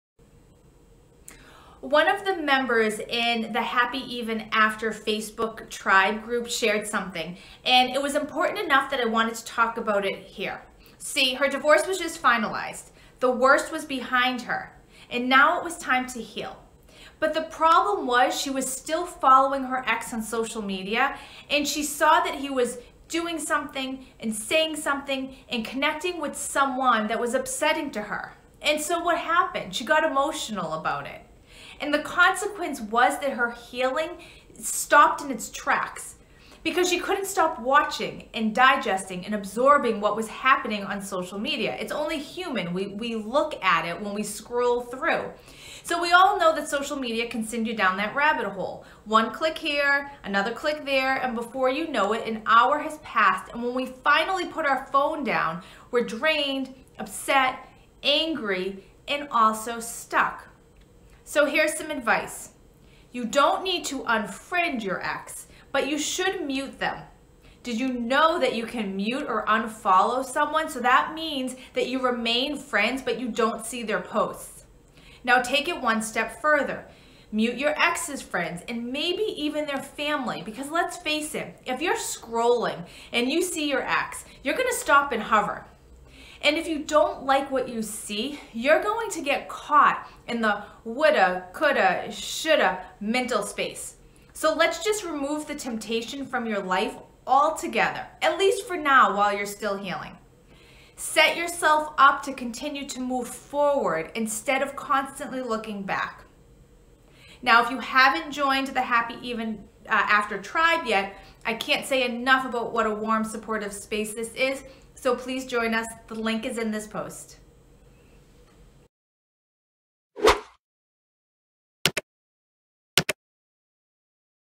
Unfollow Your Ex with Ms. Renee Bauer
Should I unfollow my ex?

I recently got asked this in my Happy Even After tribe by a member who kept seeing her ex on social.

Seeing your ex on social media can be triggering after a divorce. But it doesn’t have to be. You can maintain healthy boundaries and still be friends with them.

You don’t need to unfriend them but you can mute or unfollow them if you are spending too much energy on keeping tabs on them during your healing phase.